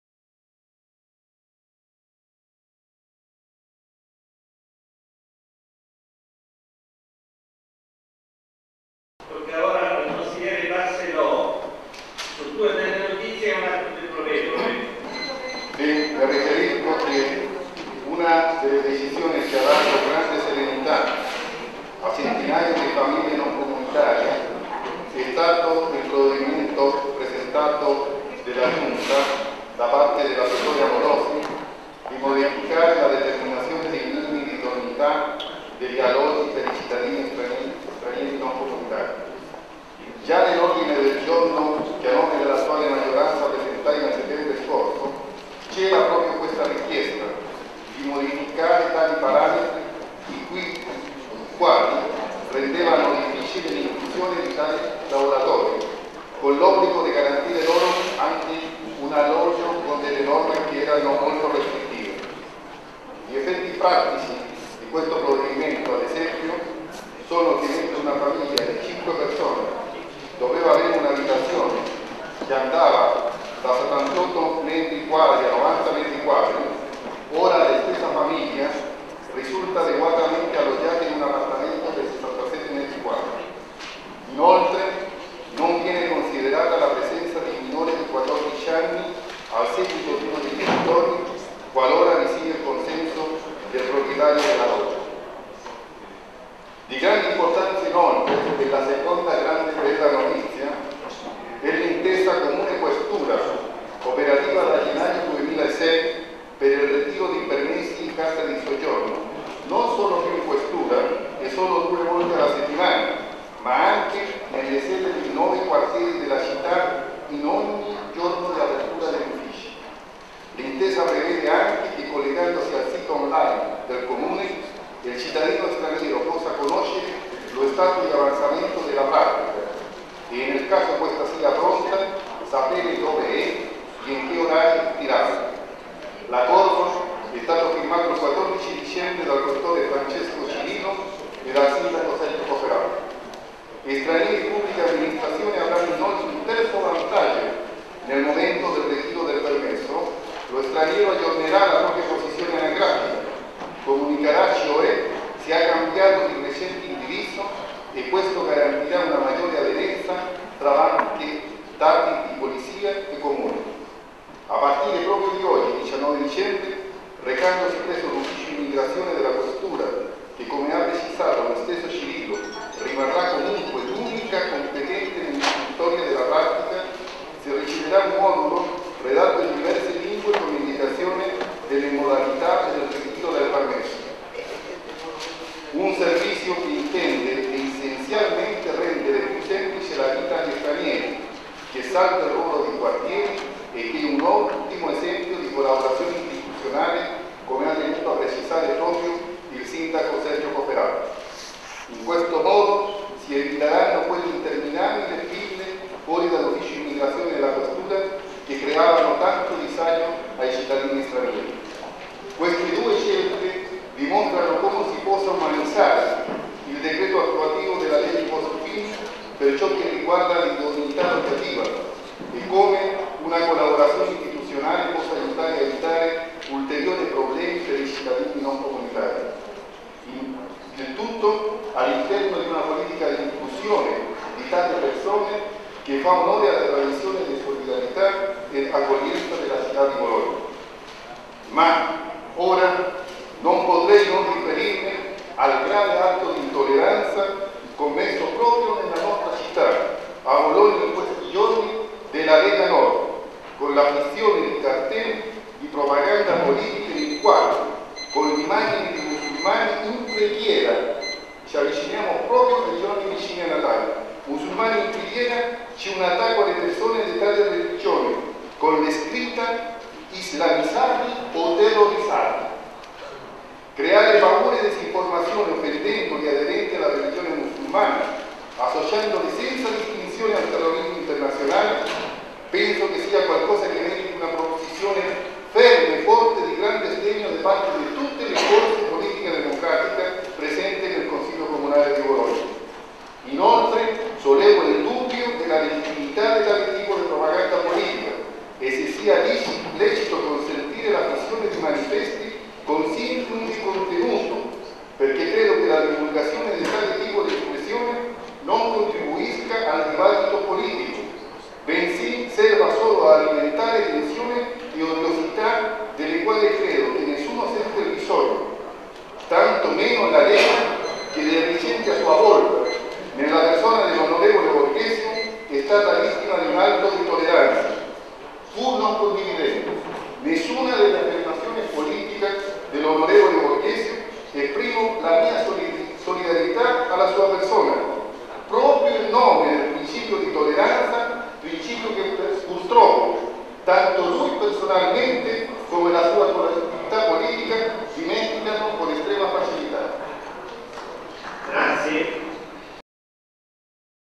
Indignazione per il nuovo cartellone pubblicitario della Lega... se ne parla in Consiglio Comunale
NADiRinforma
19 dicembre 2005 - a seguito della denuncia politica nei confronti della campagna pubblicitaria diffamatoria, promossa dalla Lega Nord, il Consigliere Comunale Leonardo Barcelo è intervenuto in sede Consigliare